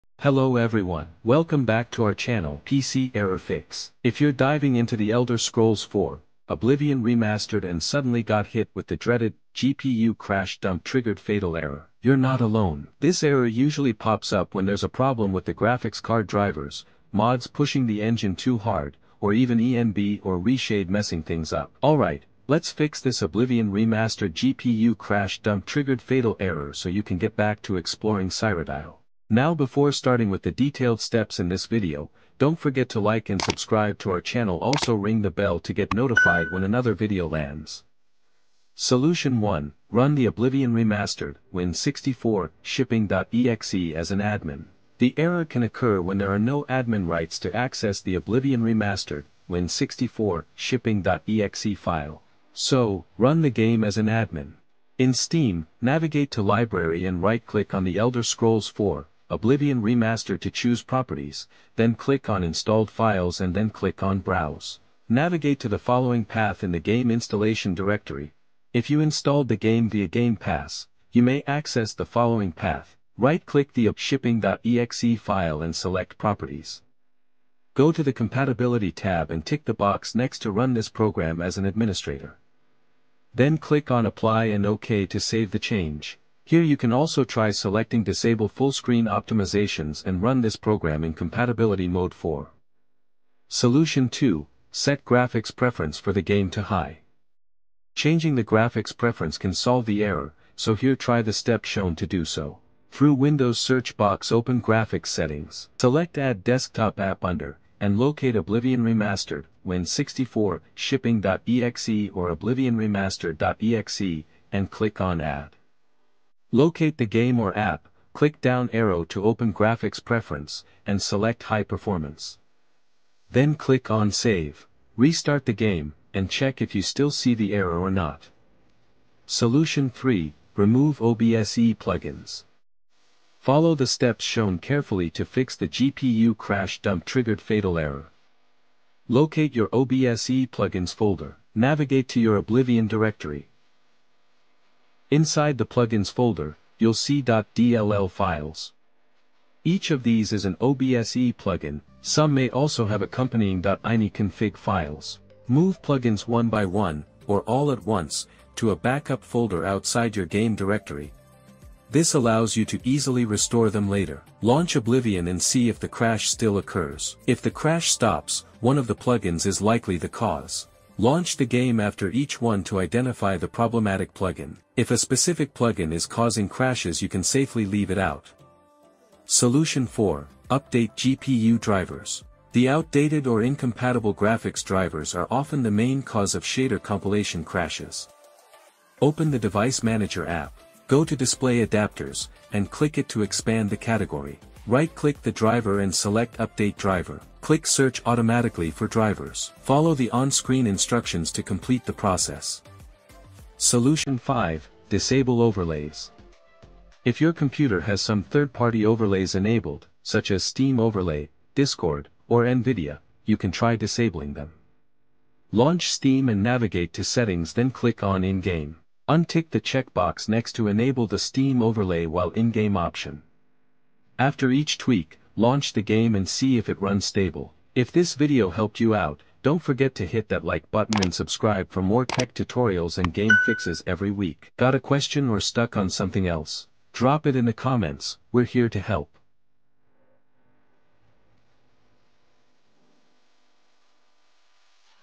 Full Fixes Oblivion Remastered GPU Crash Dump Triggered Fatal Error | PC Error Fix
If you're experiencing the Oblivion Remastered GPU Crash Dump Triggered Fatal Error, this video will show you how to fix it on your PC quickly and easily. No more frustrating crashes – just follow our step-by-step guide!

Say goodbye to the error and enjoy a smooth gaming experience!

Key Moments:
0:00 Intro
0:40 Solution 1: Run the OblivionRemastered-Win64-Shipping.exe as an Admin
1:34 Solution 2: Set Graphics Preference for the Game to High
2:09 Solution 3: Remove OBSE Plugins
3:01 Solution 4:  Update GPU Drivers
3:24 Solution 5: Disable Overlays